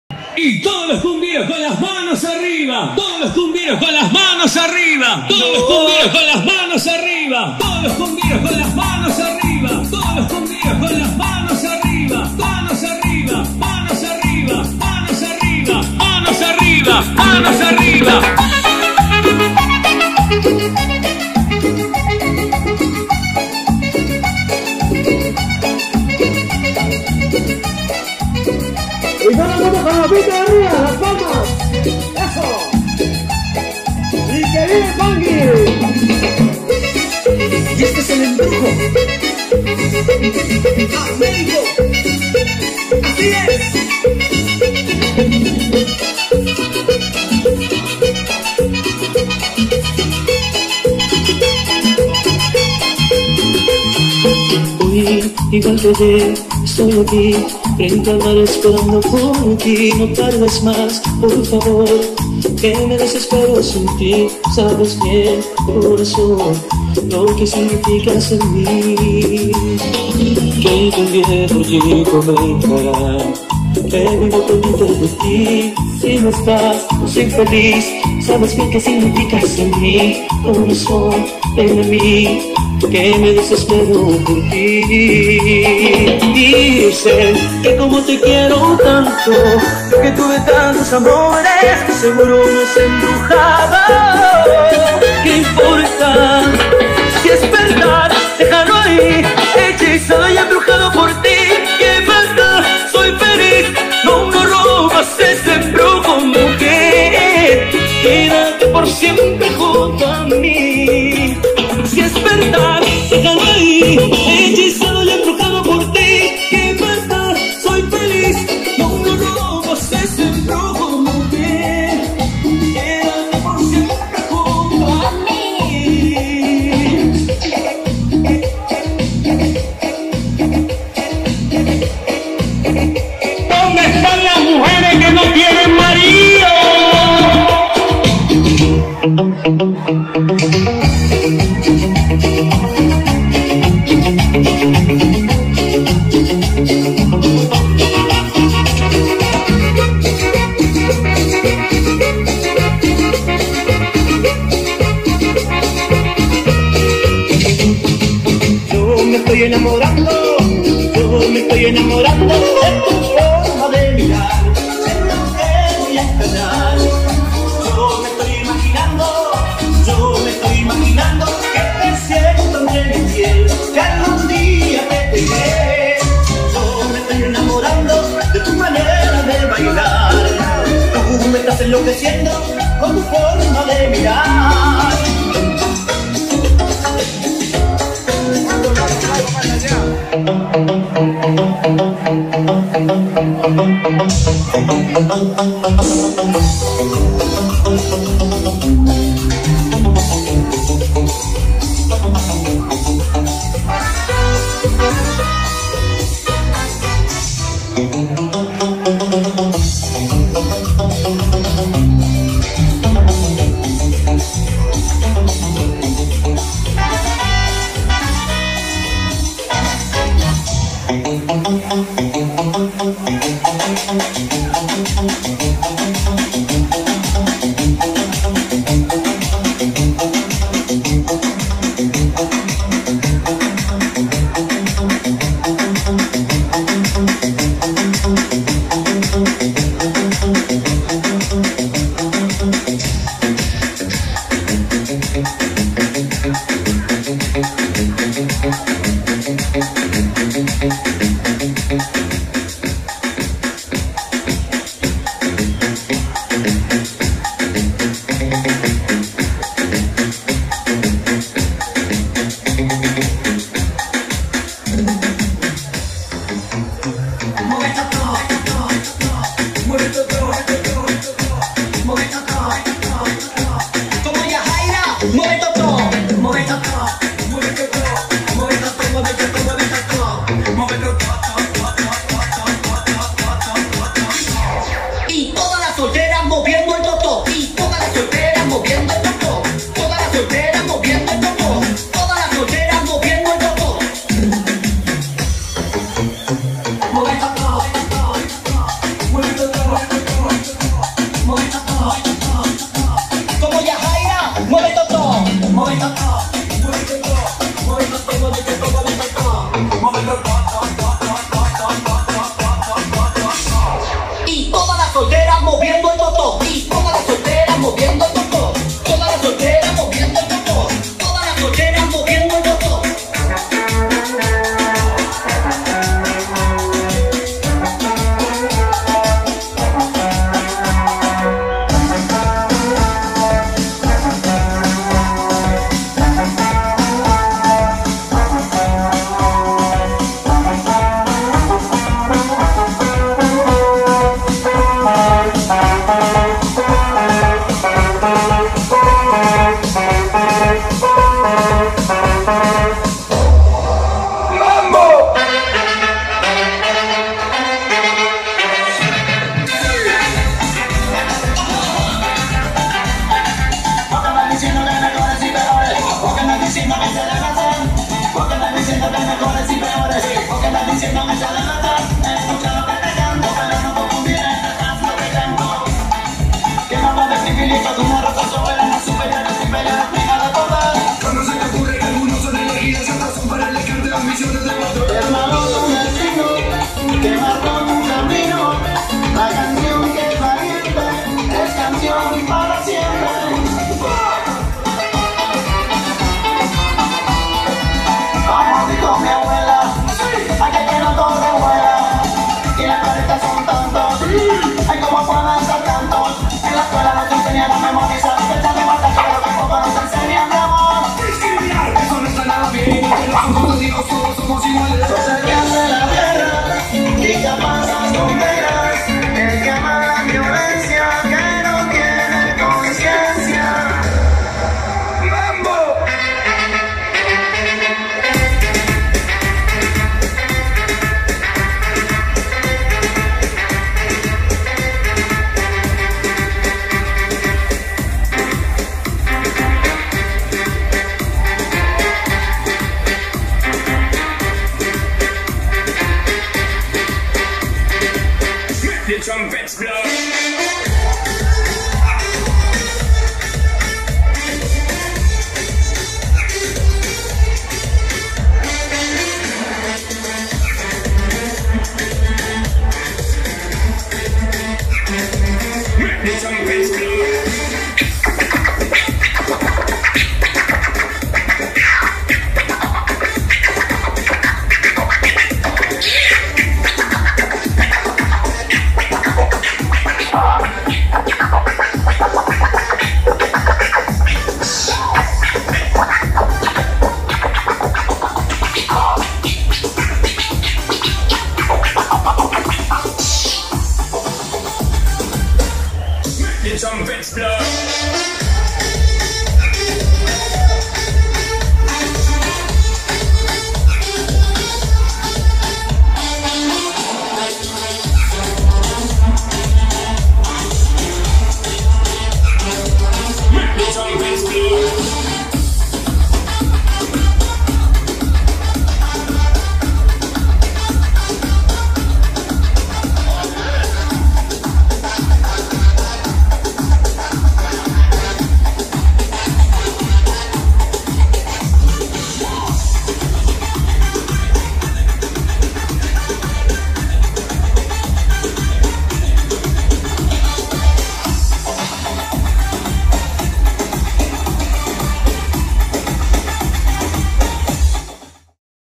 Jhostyn Villareal en el Oriente Ecuatoriano sector el Pangui
Con ustedes unos de los mejores Djs del Ecuador
Disculparan las fallitas y el Audio tambien fue grabado desde el celular.
Creditos: Jhostyn Villarreal un maestro en mesclas